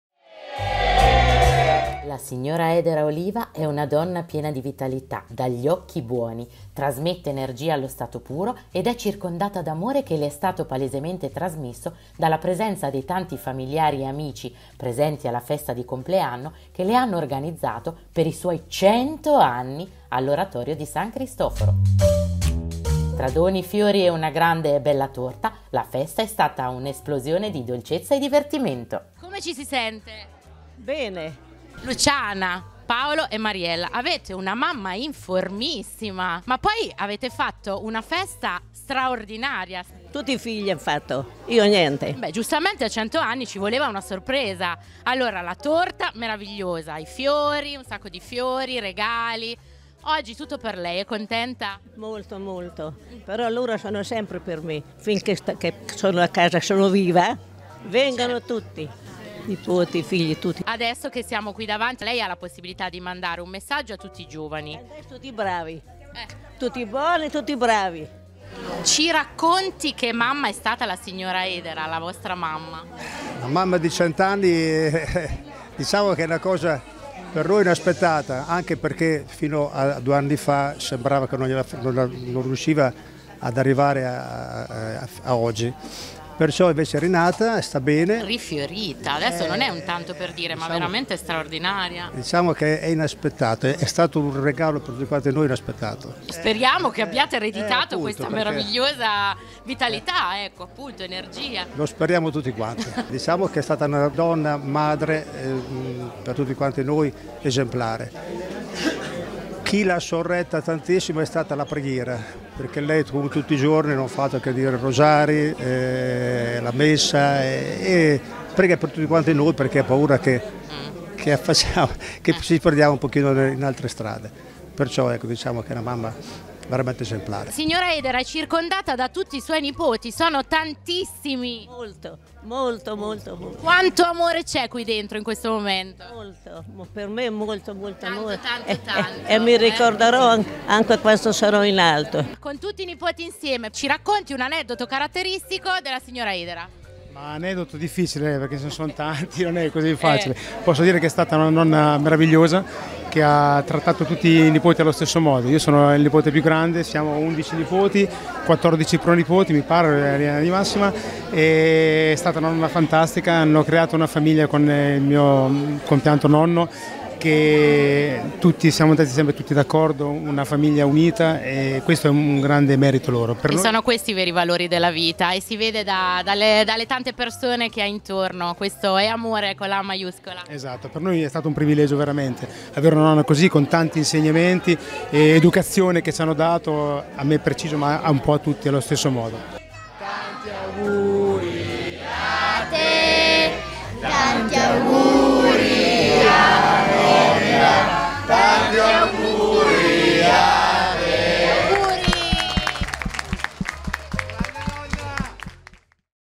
Edera Oliva compie 100 anni: festa a San Cristoforo di Fano
La signora Edera Oliva ha compiuto 100 anni: un grande traguardo festeggiato insieme a parenti e amici.